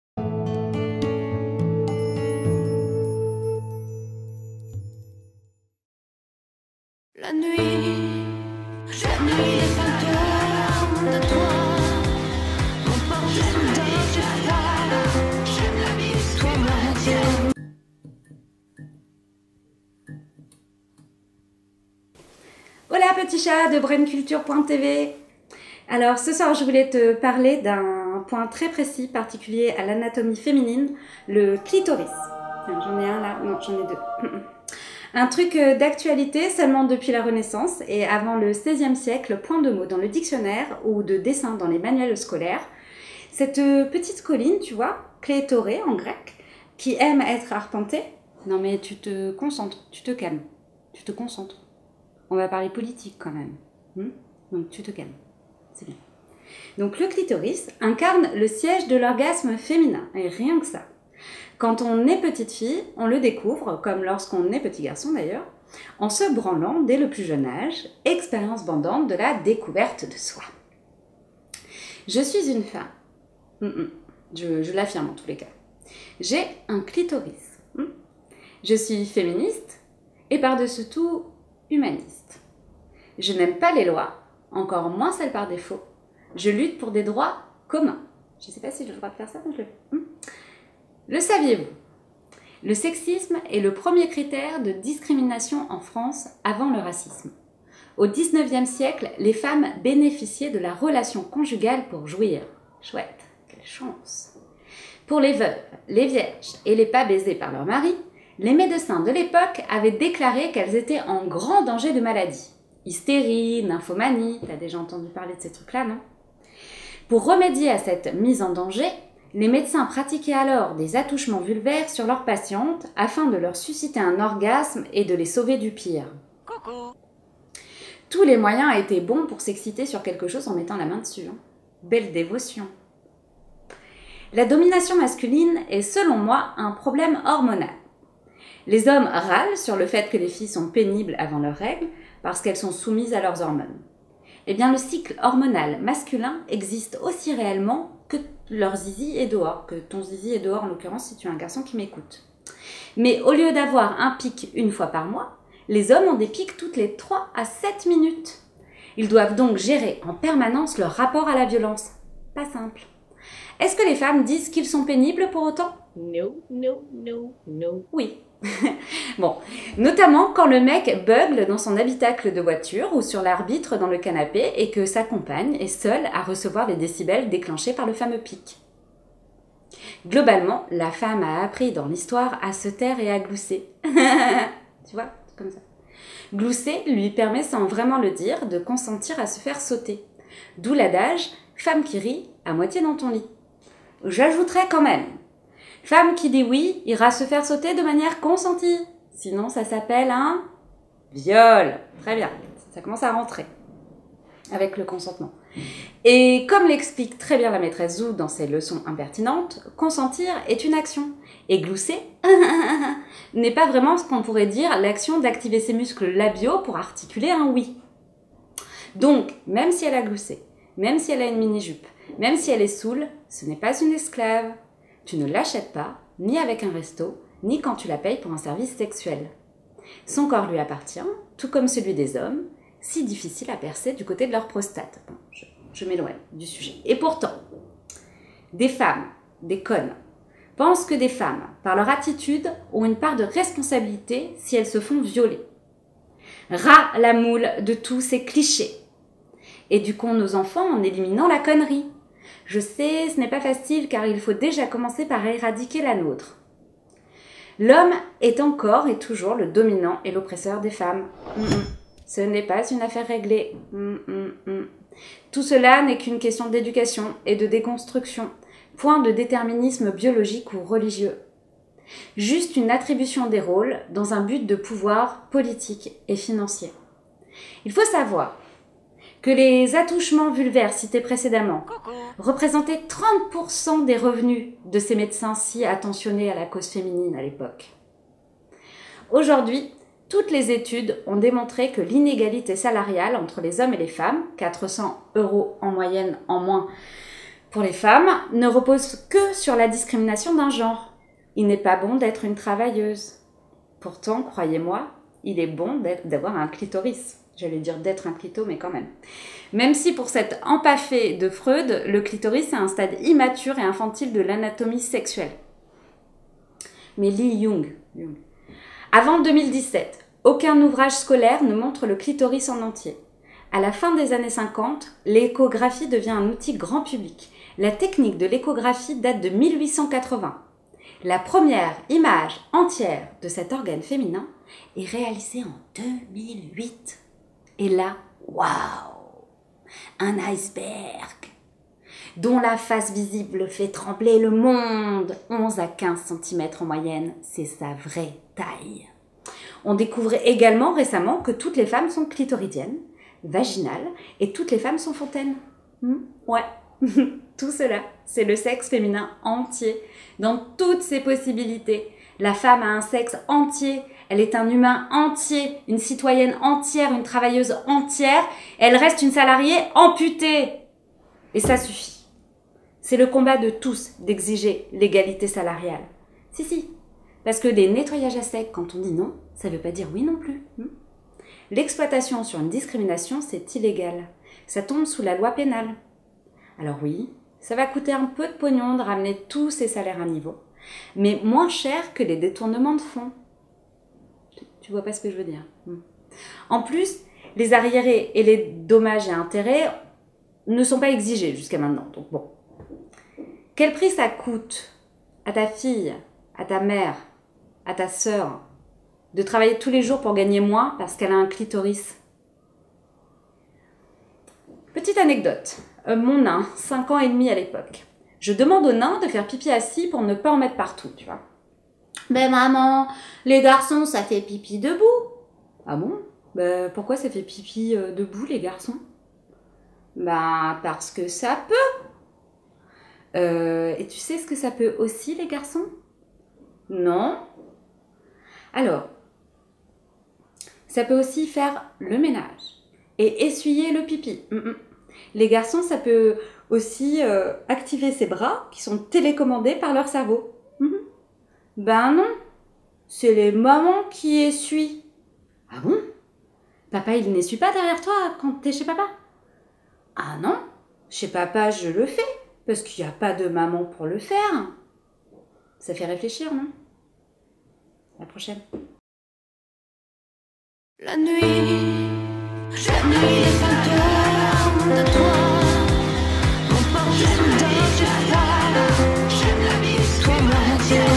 Charlotte parle aux Charlots S01Ep02 "Le clitoris"
Aujourd'hui Charlotte te parle du patriarcat et du siège,  trop méconnu, de l'orgasme féminin ...

Brainculture.tv est une ouebTv où vous pourrez trouver tout un tas d'autres contenus liés à toutes les cultures